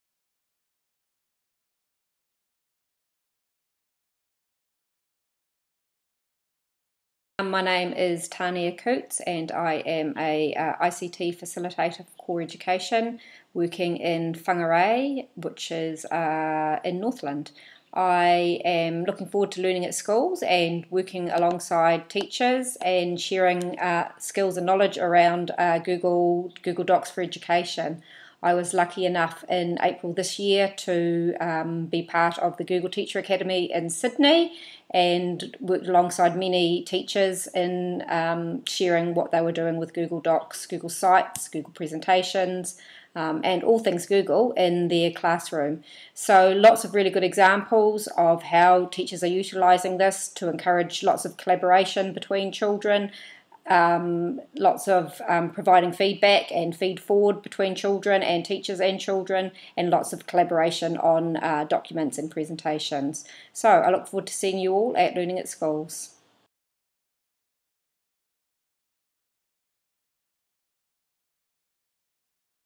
Learning@School 2012 : Speakers Taster - Tania Coutts
Another video to whet your appetite for next months Learning@School 2012 conference in Hamilton - this time Tania Coutts discussing her session on Google Docs / sites etc in education.